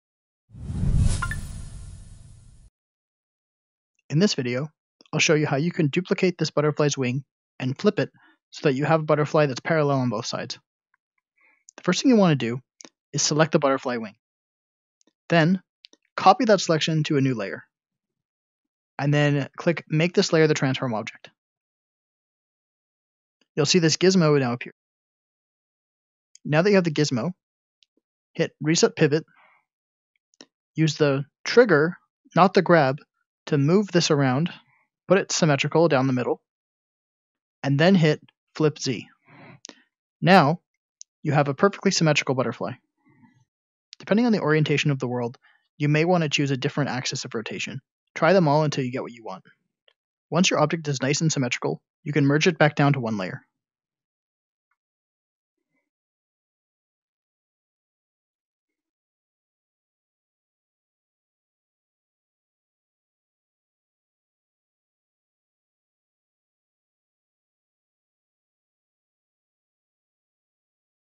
Advanced Quill Tutorial - Flipping/Symmetry
This tutorial outlined how you can take a piece of your model and duplicate/flip it over an axis to create symmetry. It's a multi-step process and isn't very intuitive for beginners!